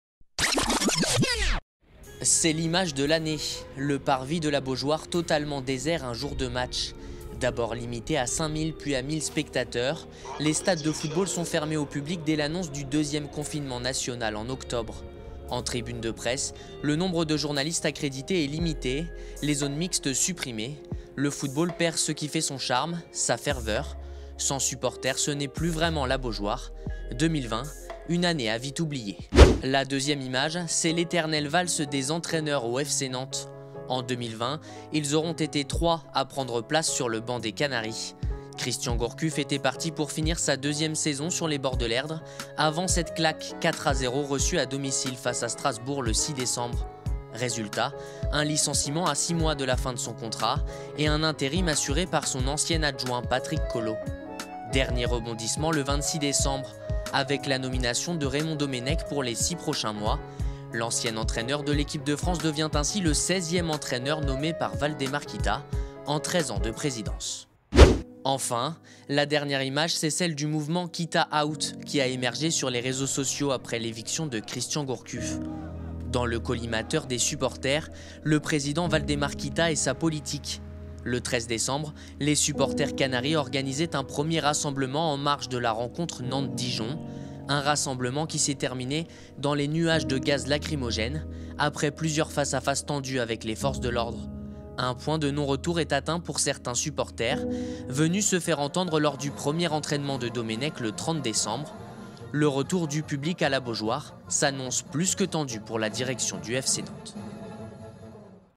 Football : les temps forts de 2020 (30/12/20)
Reportage diffusé sur Télénantes le 30 décembre 2020. Maxime Buchot.